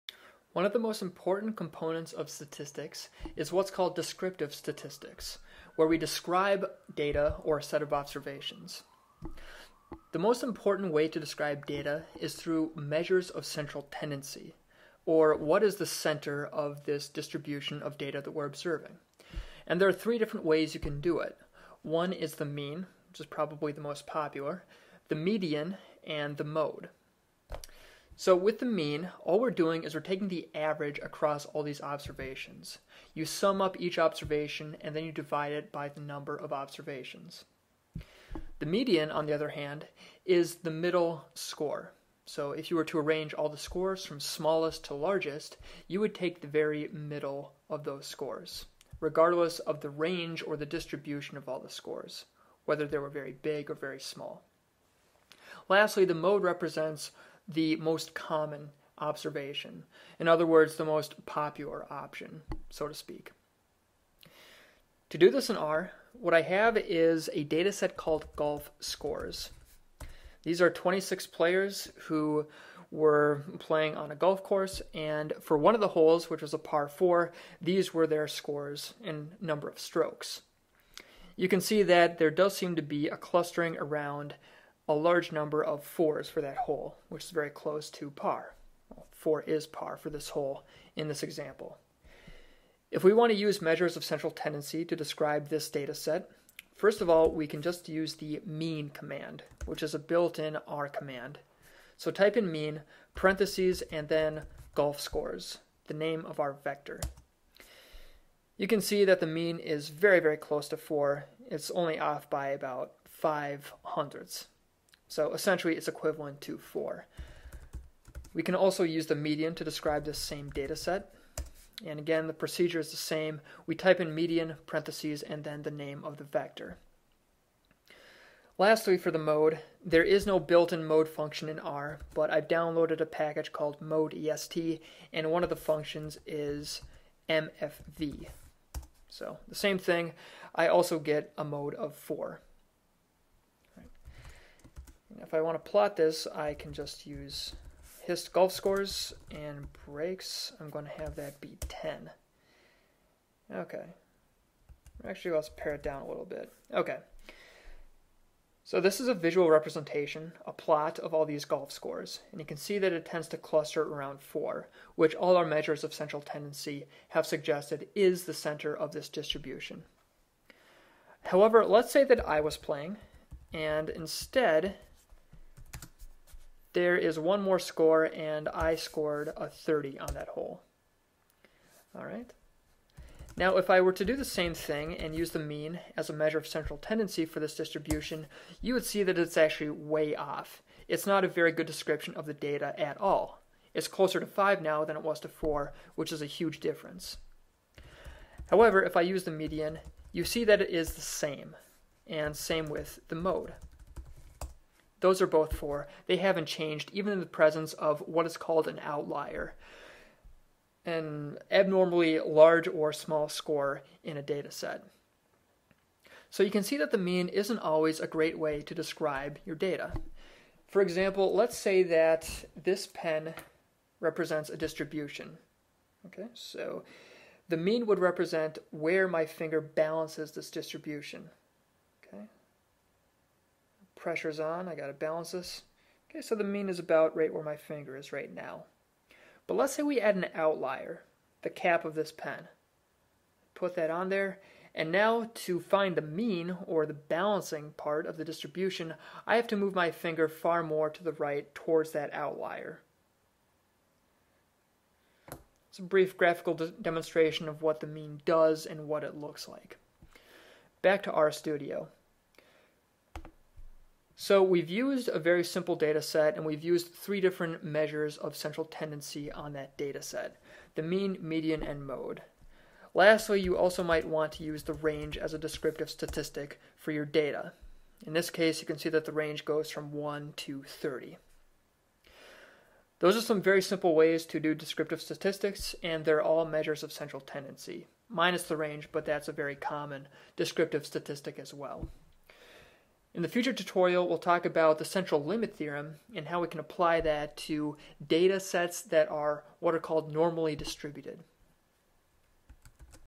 Central Tendency in R: Mean, Median, and Mode
Demonstration of the mean, median, and mode in R